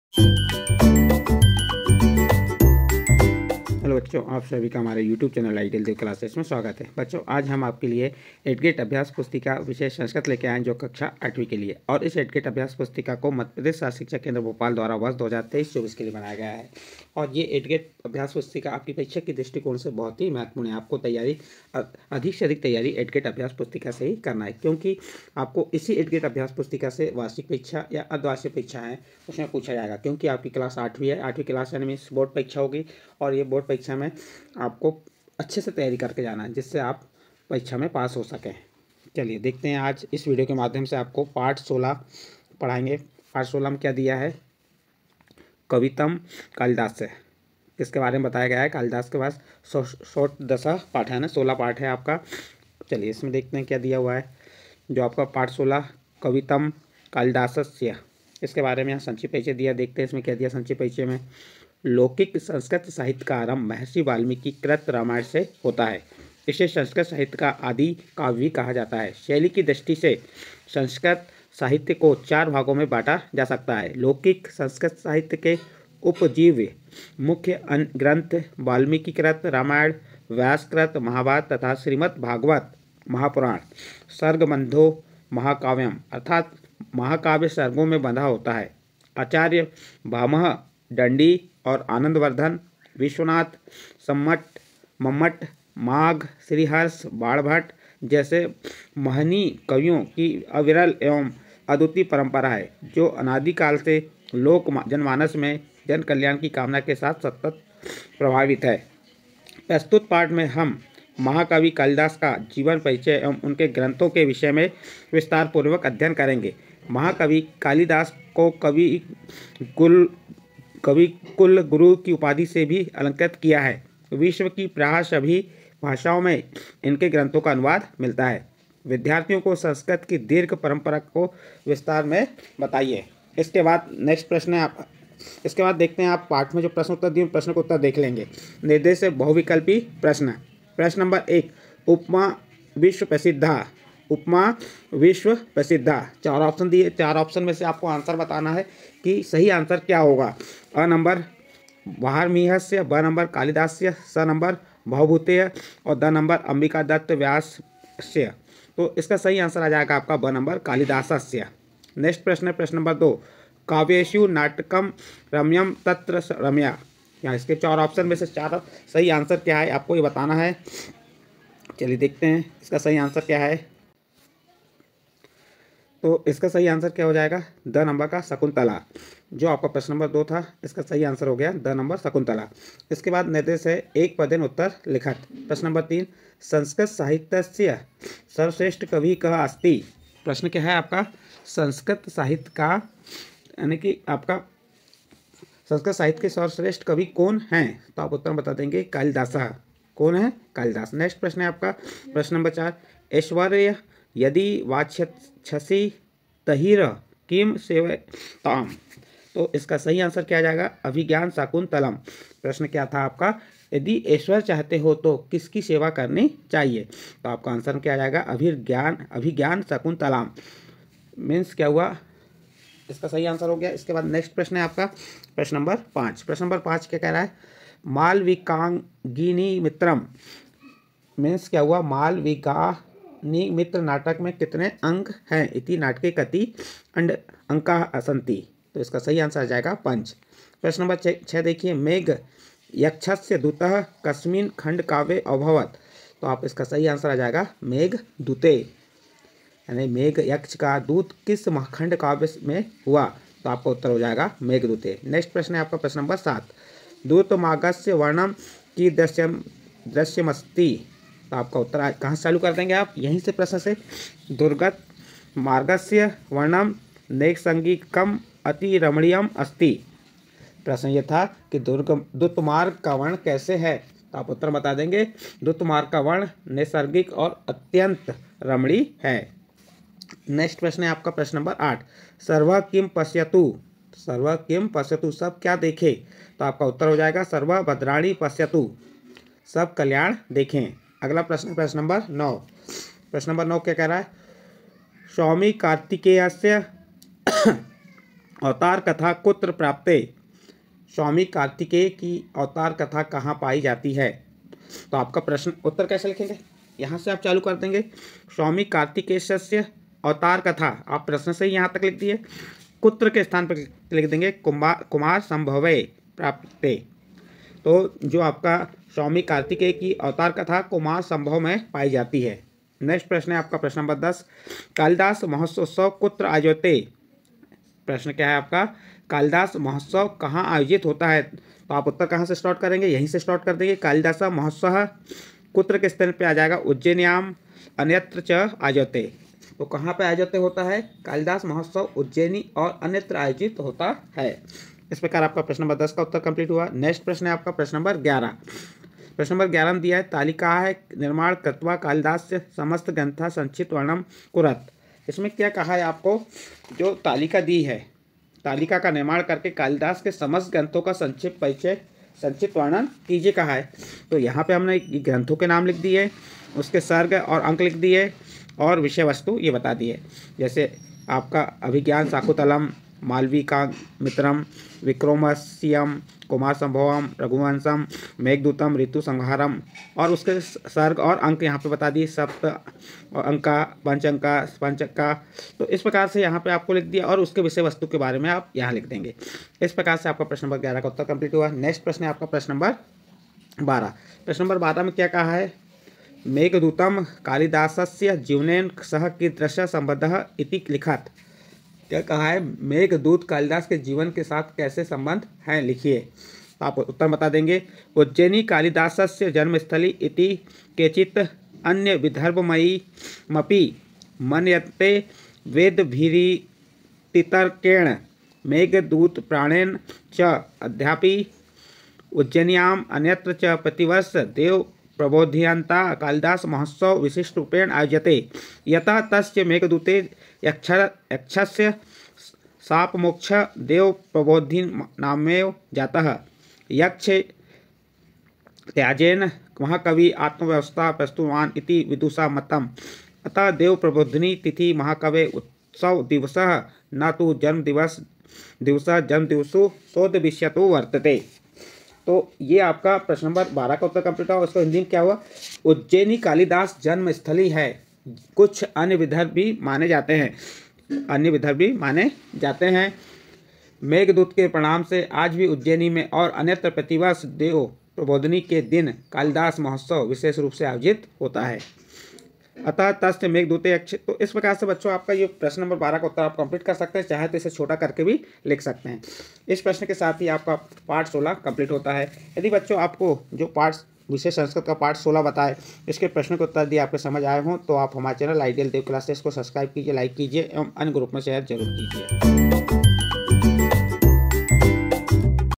एट ग्रेड अभ्यास पुस्तिका विषय संस्कृत कक्षा-8वीं षोडश: पाठः कवित्वं कालिदासस्य सत्र 2023-24
I hope understand this video

कक्षा-8वीसंस्कृत
षोडश:पाठःकवित्वंकालिदासस्य